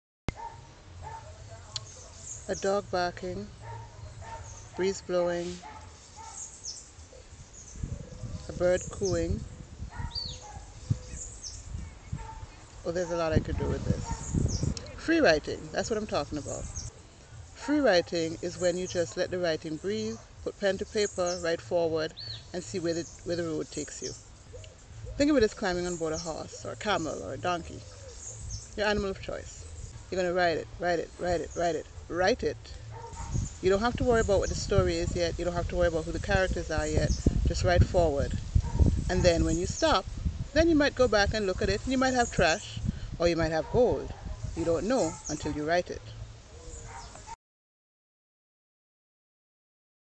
Wadadli Pen Writing Tip - Just Ride
Wadadli Pen writing challenge 2023 is open for submissions to May 31st 2023. Visit wadadlipen.wordpress.com for details.